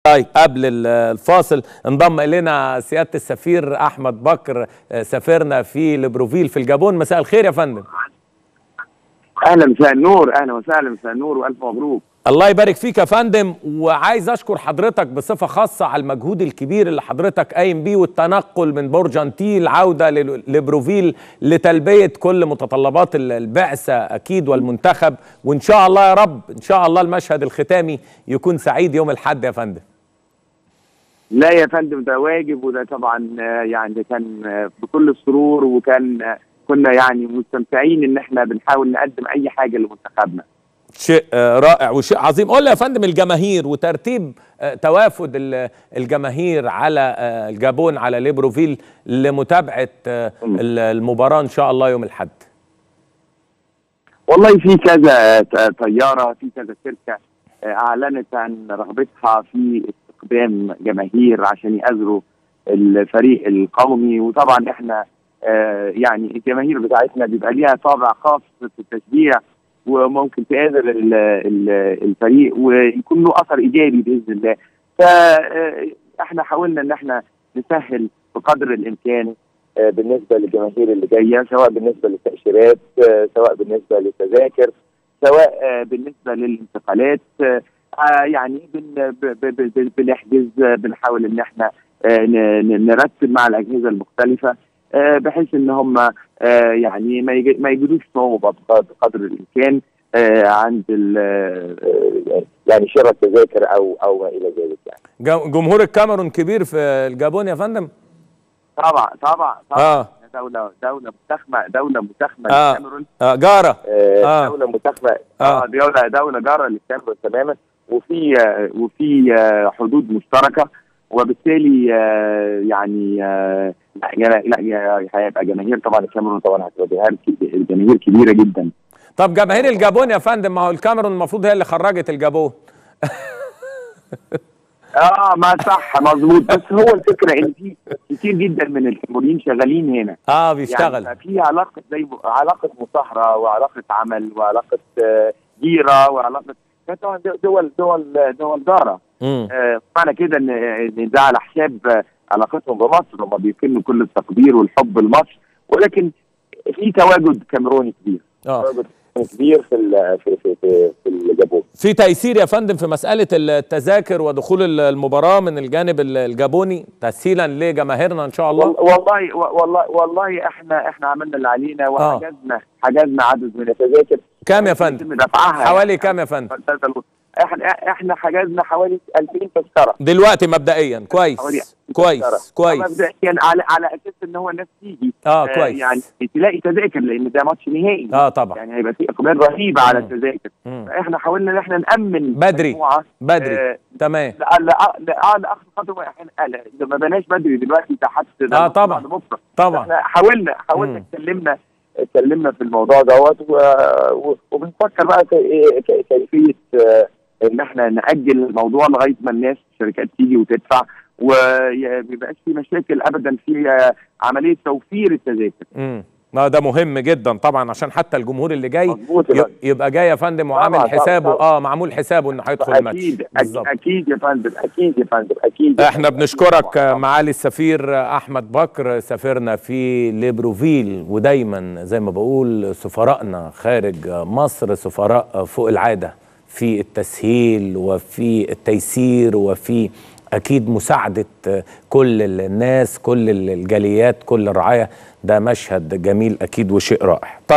Can 2017 - سفير مصر في الجابون يكشف الترتيبات للجماهير المصرية لمشاهدة النهائي الافريقي
هاتفيا السفير احمد بكر سفير مصر في الجابون
التردد: 11449.
أفقي.
القمر: نايل سات.
معامل الخطأ: 5/6.